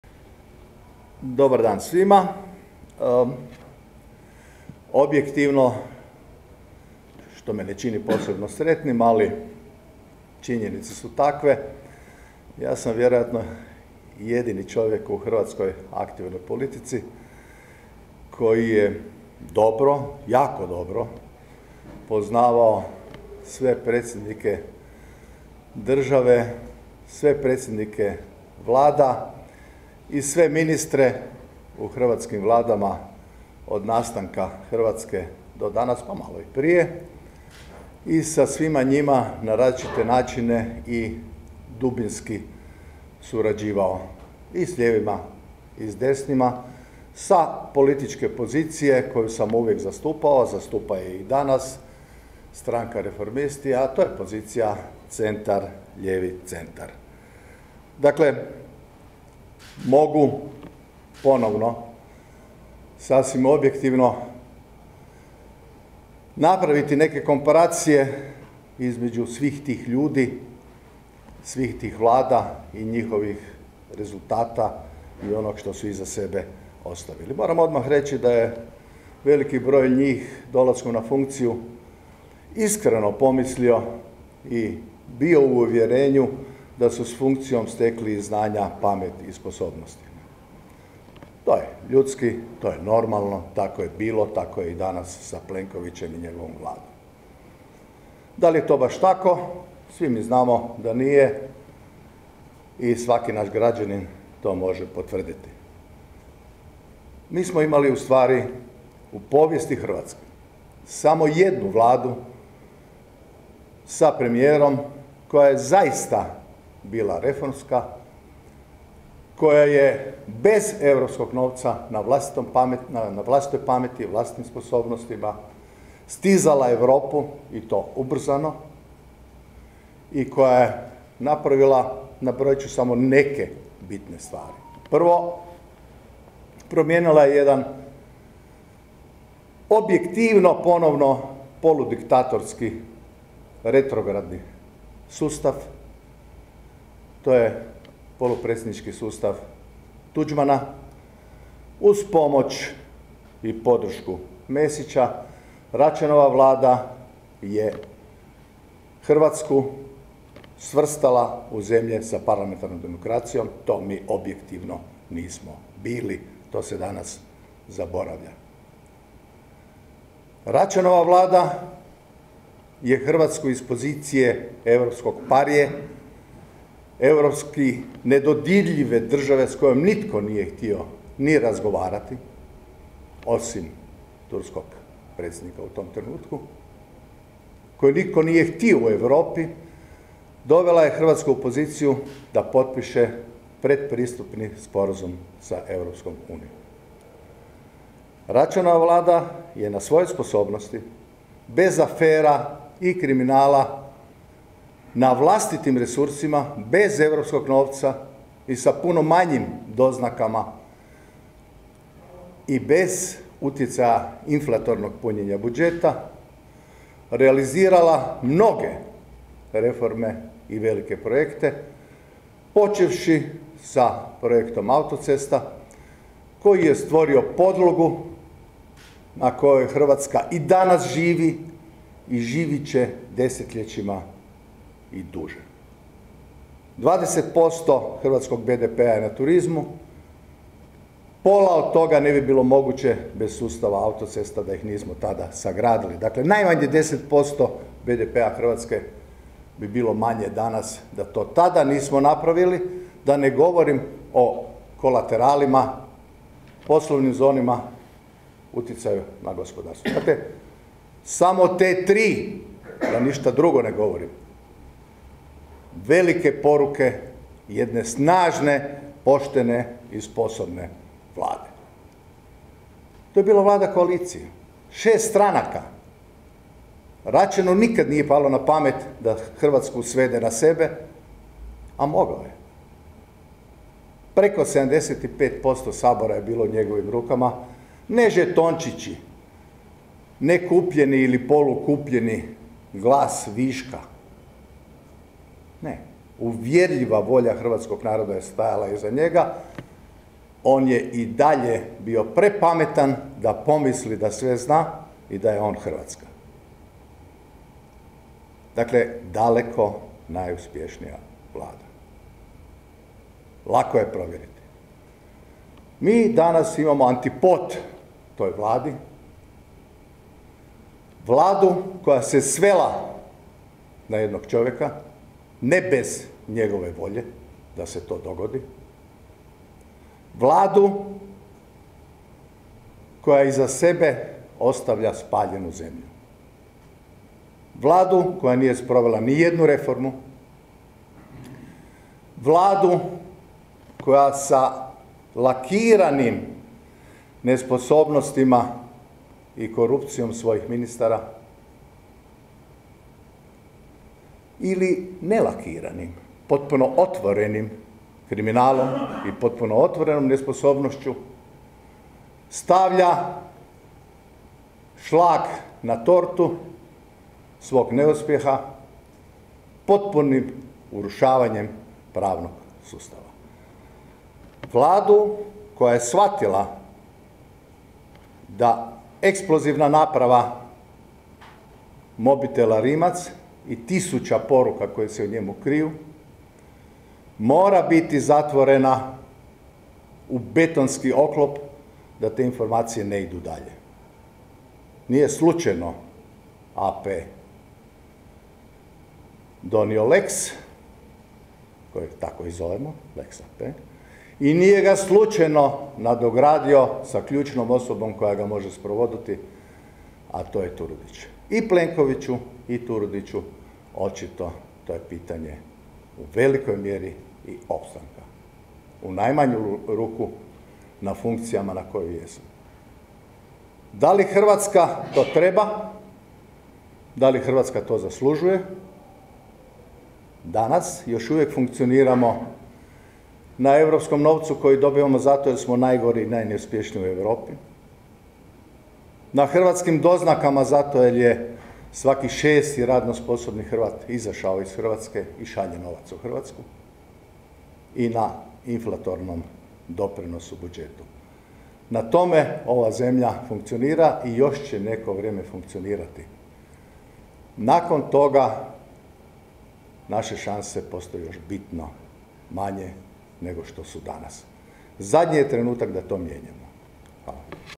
Čačić: "Mi danas imamo Vladu koja iza sebe ostavlja spaljenu zemlju"
Na konferenciji za medije Kluba zastupnika Fokusa i Reformista i Kluba zastupnika IDS-a održanoj u Saboru o temi "Zahtjev oporbe za raspuštanjem Sabora" govorio je Radimir Čačić.